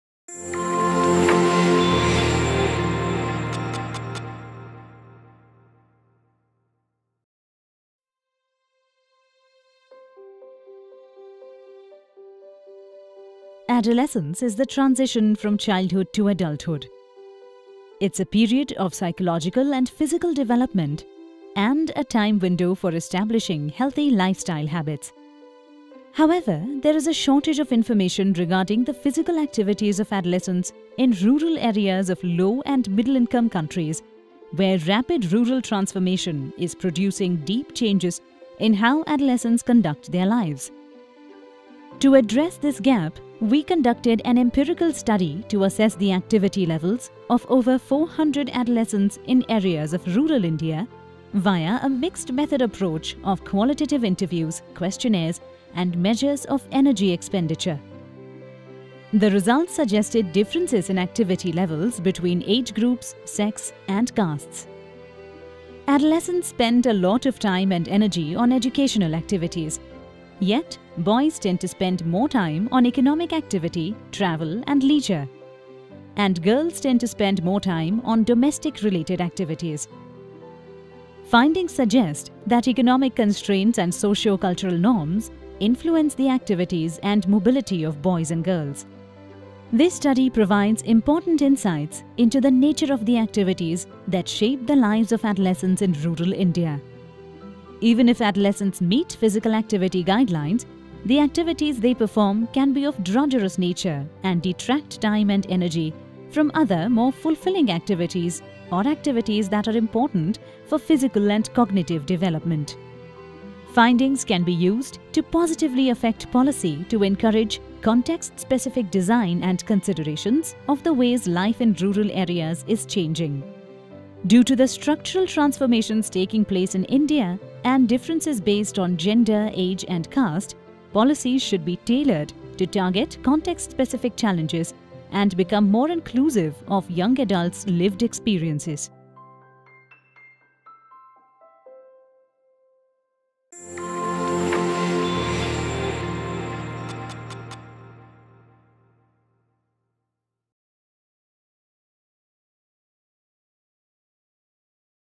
What matters in physical adolescent activity – quality or quantity?
What matters in physical adolescent activity – quality or quantity? These are the findings from a mix-methods study in rural Telangana.